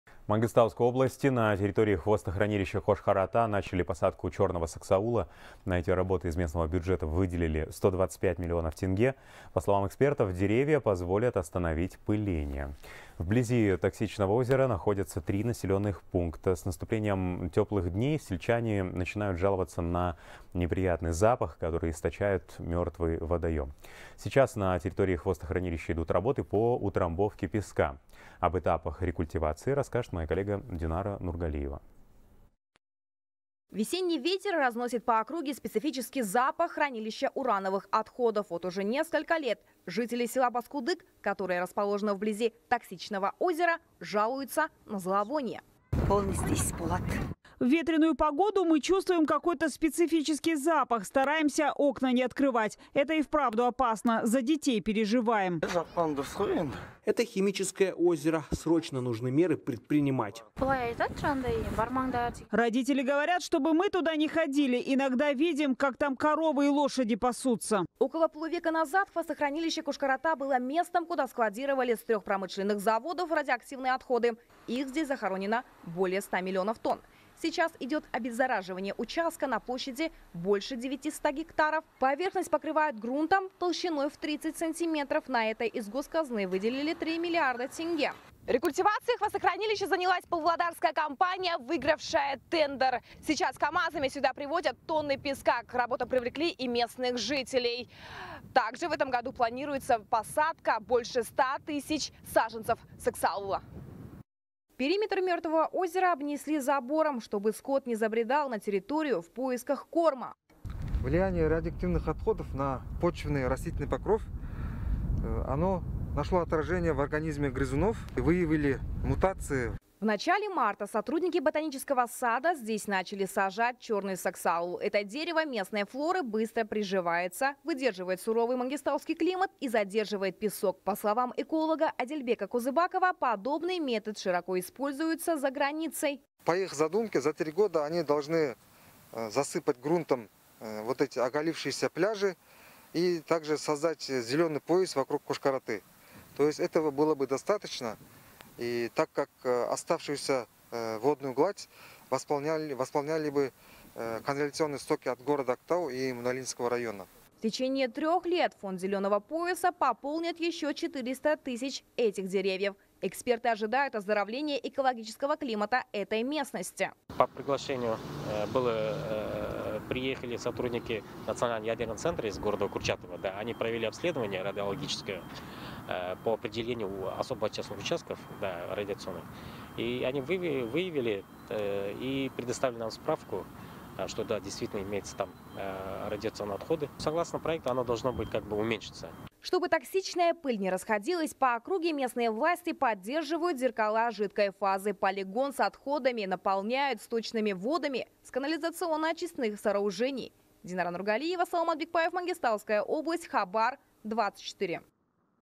Чёрный саксаул сажают вокруг хвостохранилища Кошкар ата
В Мангистауской области на территории хвостохранилища Кошкар Ата начали посадку черного саксаула.